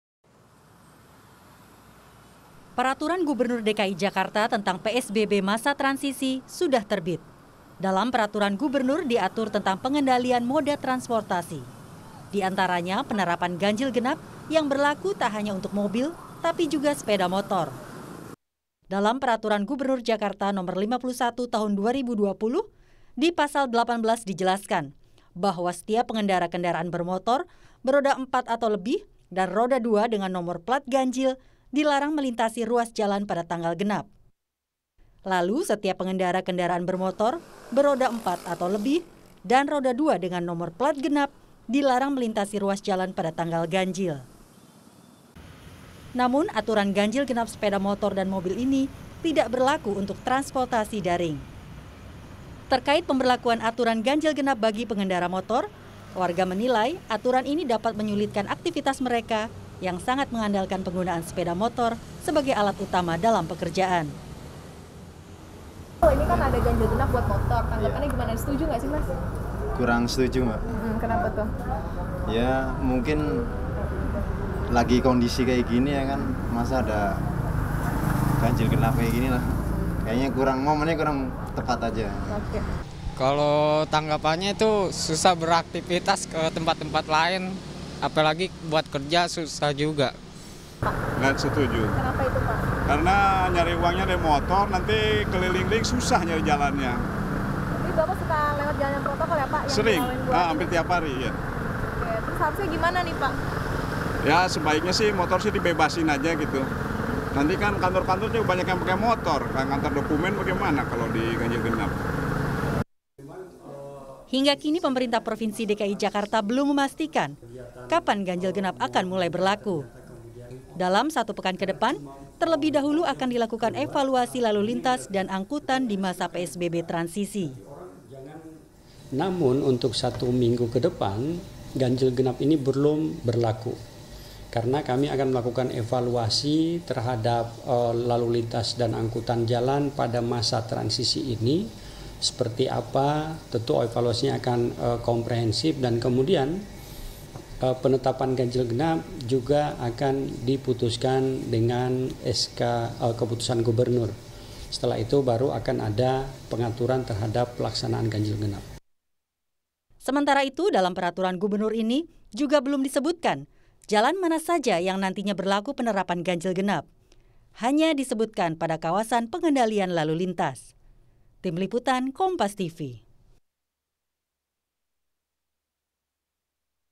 PSBB Transisi, Pemprov DKI Belum Putuskan Kapan Ganjil-Genap Berlaku
JAKARTA, KOMPAS.TV - Peraturan Gubernur DKI Jakarta tentang PSBB masa transisi sudah terbit.

Dalam peraturan Gubernur diatur tentang pengendalian moda transportasi.

Di antaranya penerapan ganjil genap, yang berlaku tak hanya untuk mobil, tapi juga sepeda motor.

Dalam peraturan Gubernur Jakarta nomor 51 Tahun 2020, di pasal 18, dijelaskan bahwa setiap pengendara kendaraan bermotor beroda empat atau lebih, dan roda dua dengan nomor plat ganjil, dilarang melintasi ruas jalan pada tanggal genap.

Lalu setiap pengendara kendaraan bermotor beroda empat atau lebih, dan roda dua dengan nomor plat genap, dilarang melintasi ruas jalan pada tanggal ganjil.

Namun aturan ganjil genap sepeda motor dan mobil ini tidak berlaku untuk transportasi online.

Terkait pemberlakuan aturan ganjil genap bagi pengendara motor, warga menilai aturan ini dapat menyulitkan aktivitas mereka, yang sangat mengandalkan penggunaan sepeda motor sebagai alat utama dalam pekerjaan.

Hingga kini, pemerintah Provinsi DKI Jakarta  belum memastikan kapan ganjil genap akan mulai berlaku.

Dalam satu pekan ke depan, terlebih dahulu akan dilakukan evaluasi lalu lintas, dan angkutan di masa PSBB transisi.

Sementara itu, dalam peraturan Gubernur ini juga belum disebutkan, jalan mana saja yang nantinya berlaku penerapan ganjil-genap.

Hanya disebutkan, pada kawasan pengendalian lalu lintas.